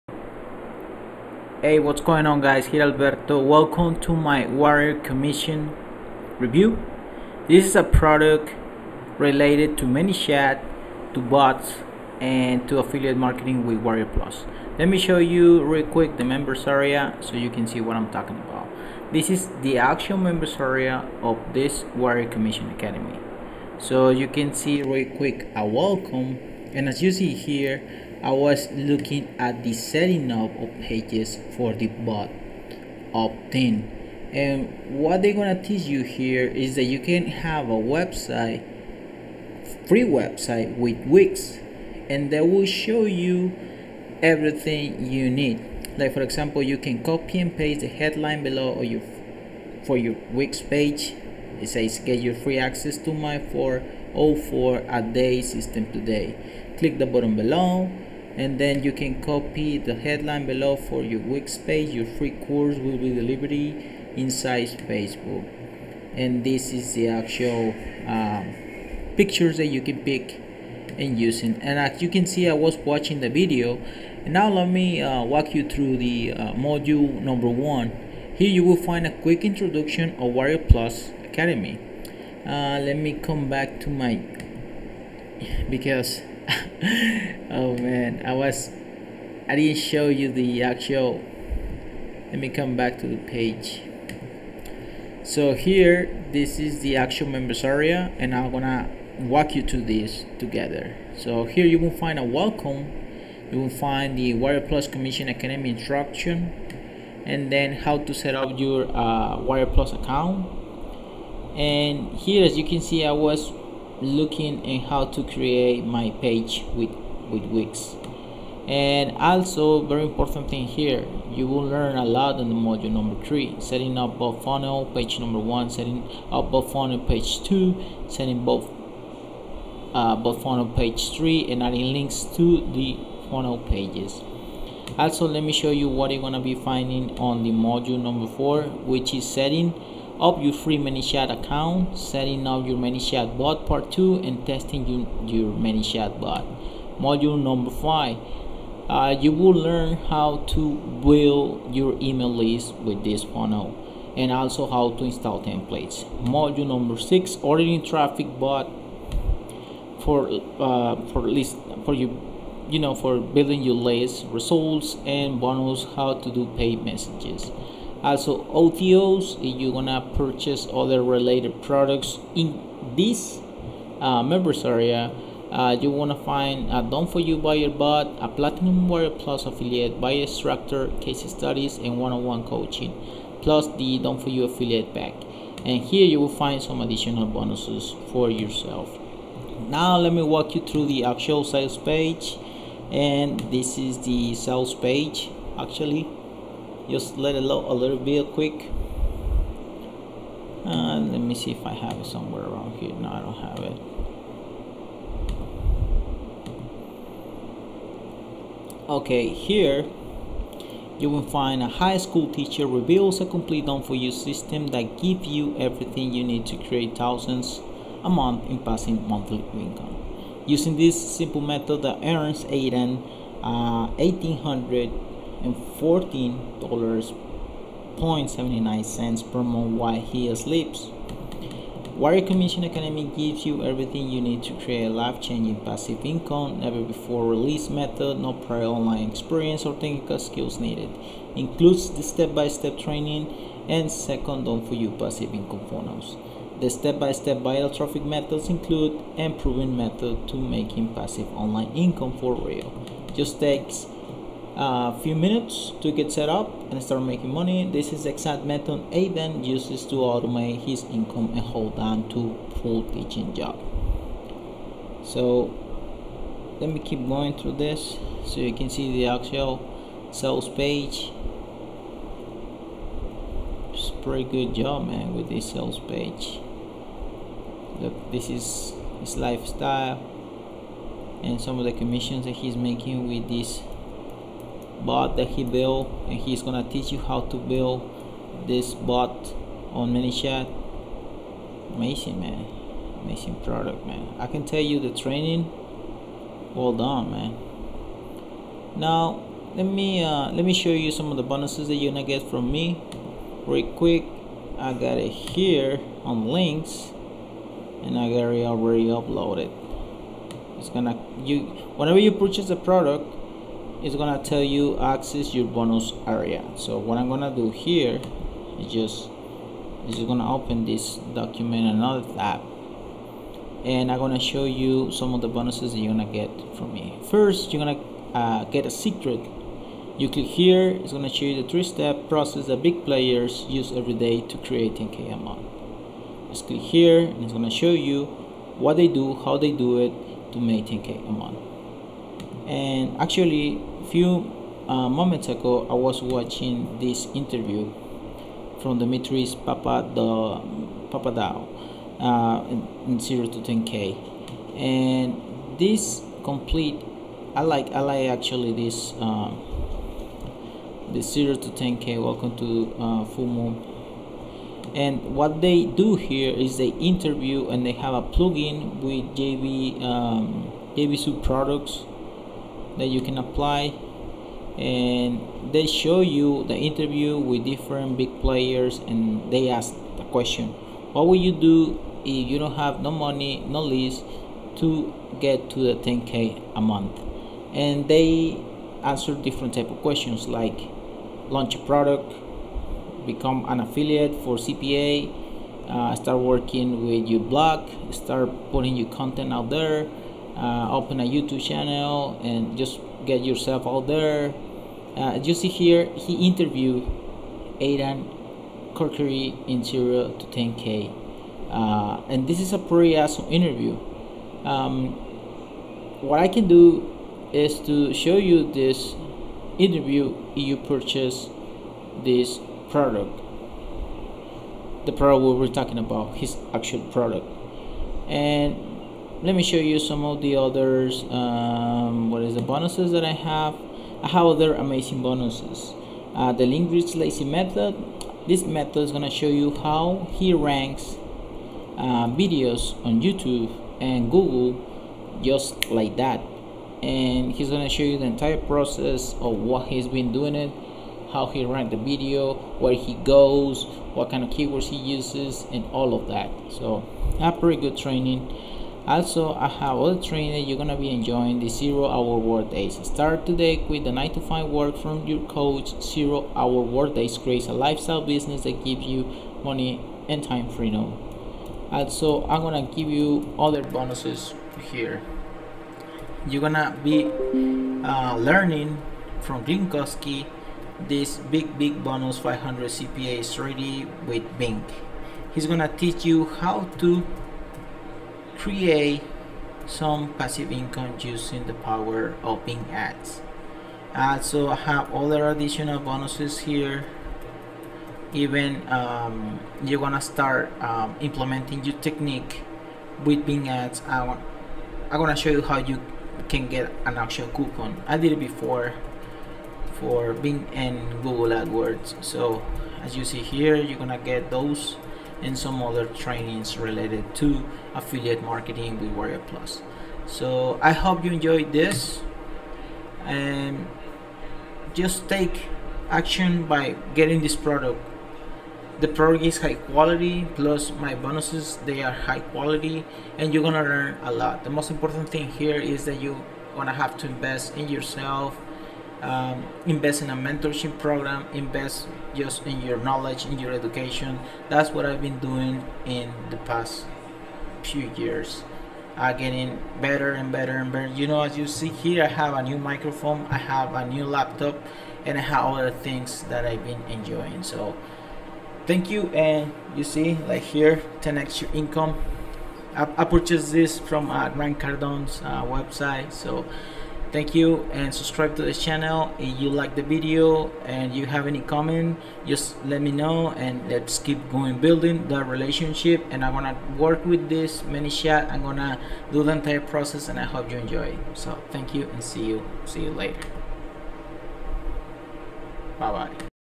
Warrior Commission Academy Review 🥶 manychat 🤖
A strategy which will show you how to grow a messenger marketing bot list of 1000+ subscriber for FREE and also shows you how to get paid as you grow it.